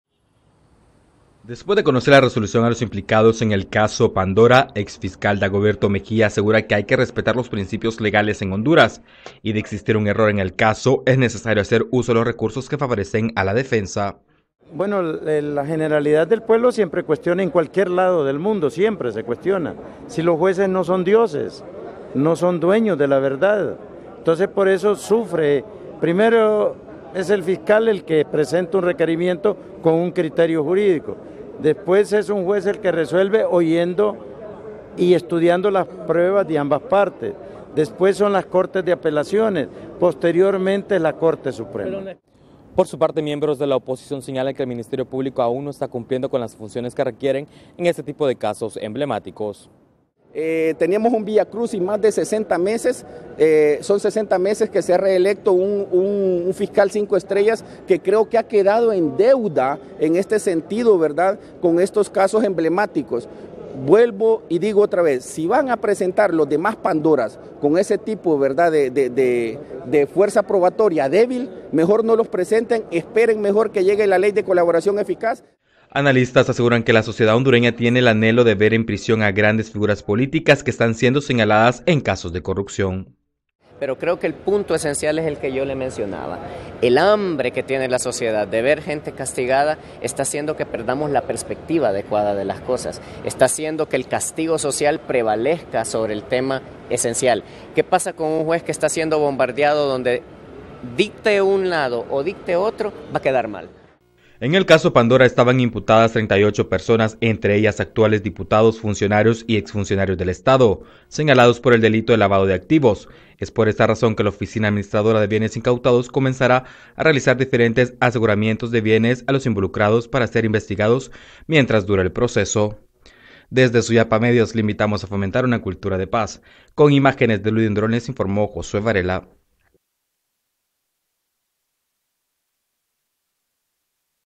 Entendidos en material legal: si juez encargada del caso cometió error en su resolución, puede ...
Entendidos en material legal, aseguran que si la juez encargada del caso cometió un error en su resolución, aun se puede aplicar la ley de manera correcta.